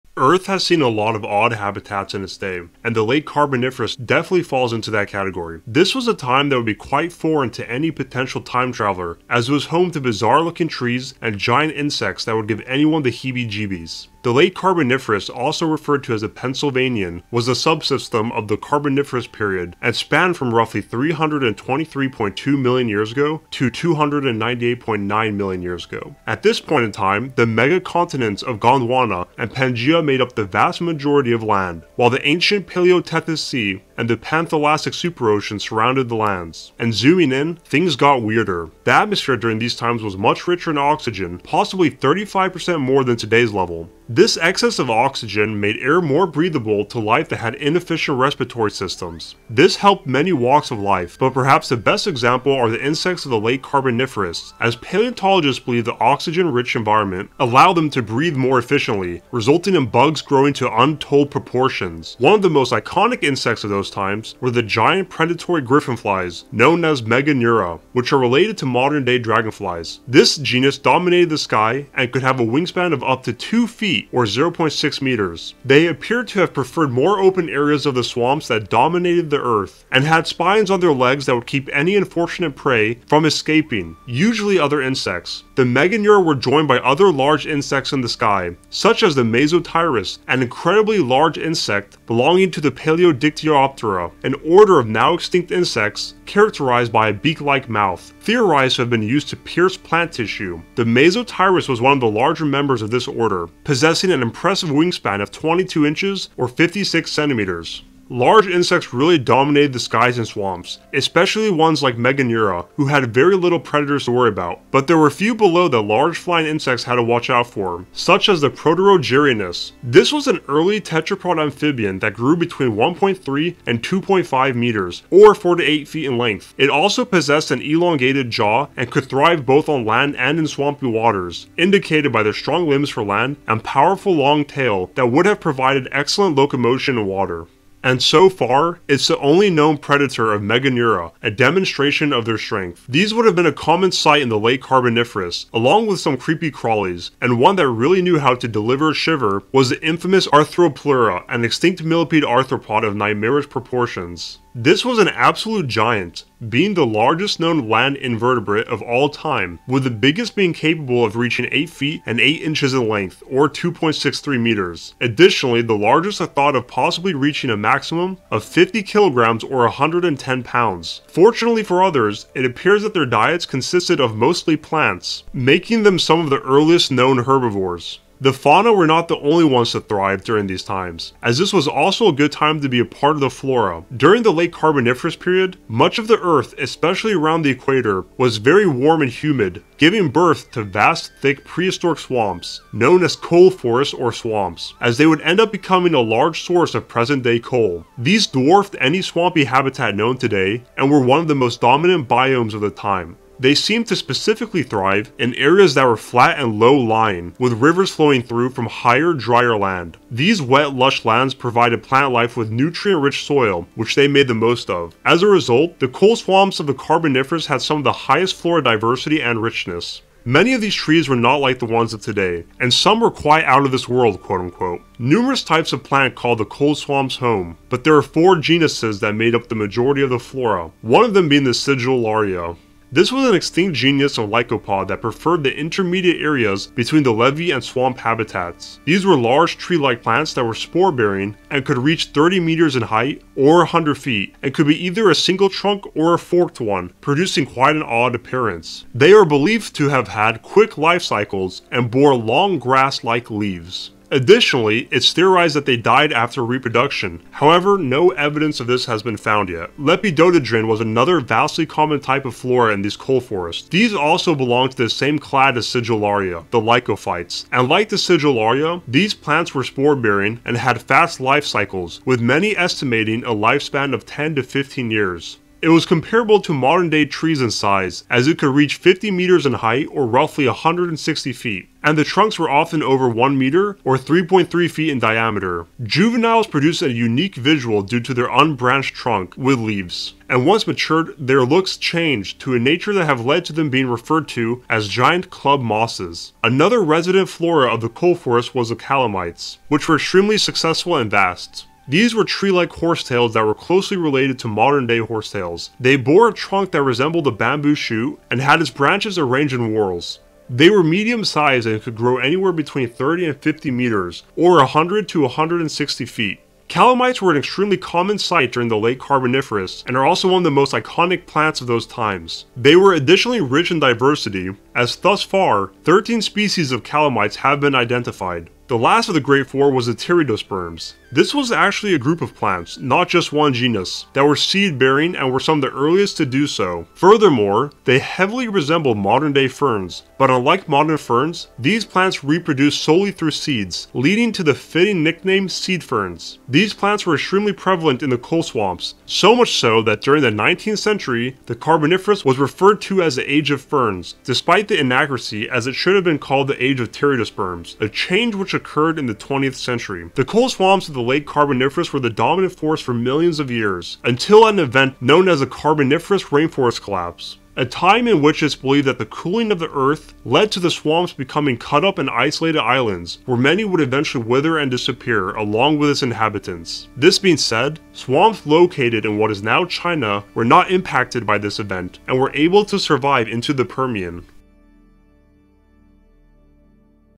Carboniferous: The Age of Giant Bugs, Amphibians, and Odd Trees
The Carboniferous was an odd time as a whole, but the Late Carboniferous was really whacky and a nightmare for most people. This was a time saturated with giant insects such as the Meganeura, as well as some serious creepy crawlies like Arthropleura, a massive millipede. It was also an extremely swampy time filled to the brim with bizarre plants, making this one of the most unrecognizable times in Earth's history.

Thumbnail:
Image Illustrated by Vitor Silva.